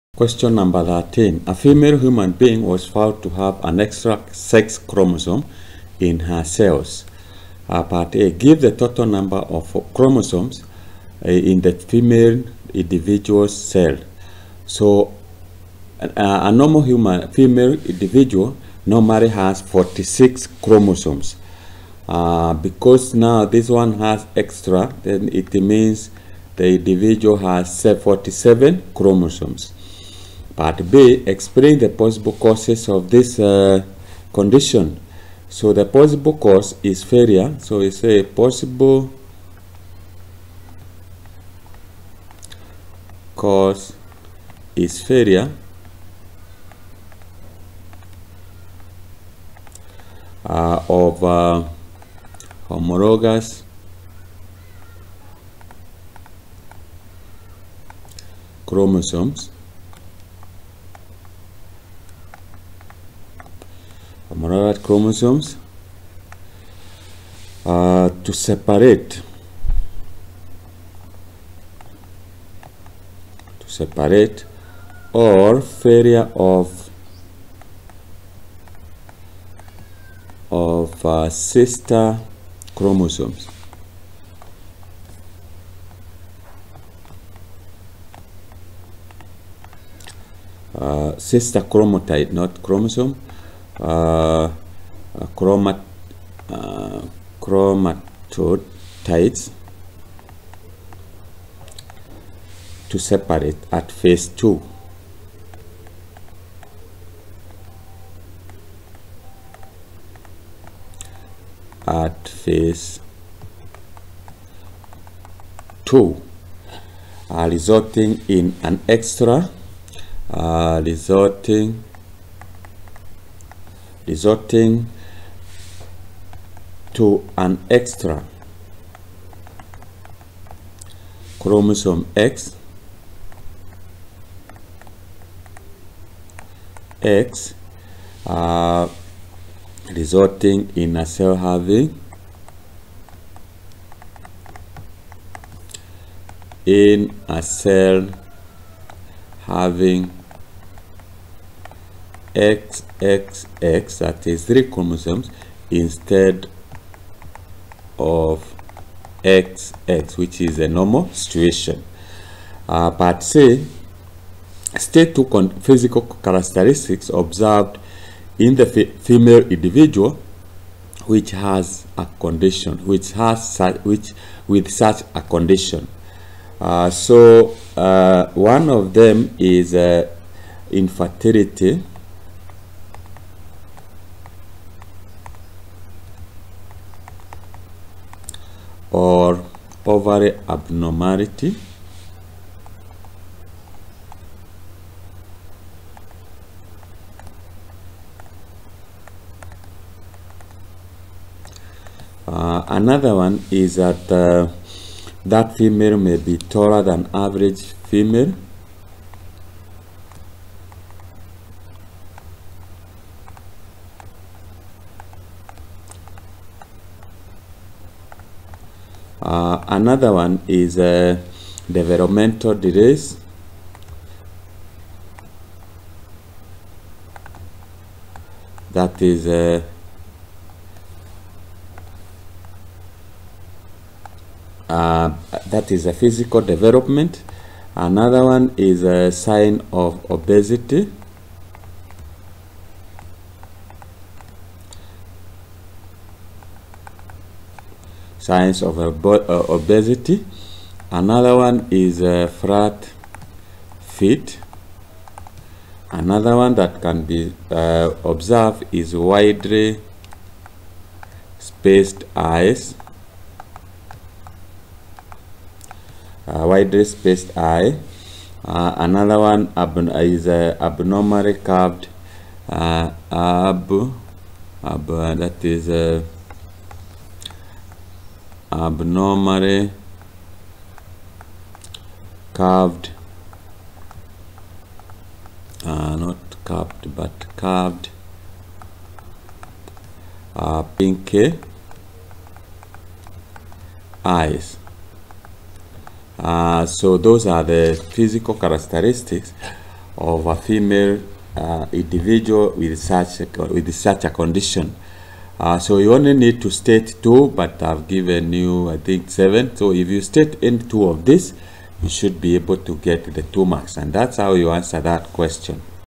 2019 KCSE Biology Paper 1 Question 13
This video explains how to correctly answer question #13 on KNEC's 2019 KCSE Biology Paper 1 exam.

13. A female human being was found to have an extra sex chromosome in her cells.

(a) Give the total number of chromosomes in the female individual’s cells.(1 Marks)

(b) Explain the possible cause of this condition.(2 Marks)

(c) State two physical characteristics observed in the female individual with such a condition.